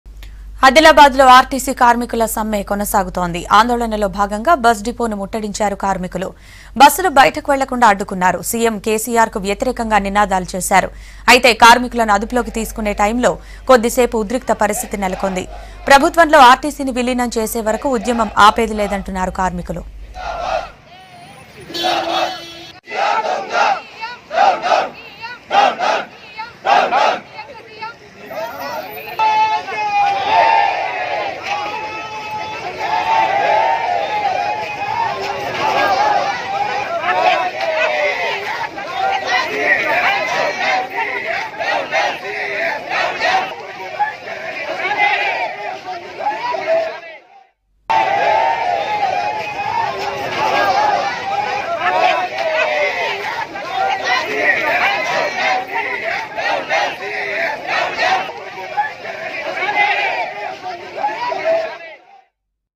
TSRTC Employees Siege Adilabad Bus Depot, Stops RTC Buses | V6 Telugu News
TSRTC Employees Siege Adilabad Bus Depot, Stops RTC Buses

బంగారు మంత్రులు జోష్... ఉద్యమ మంత్రులు కామూష్ |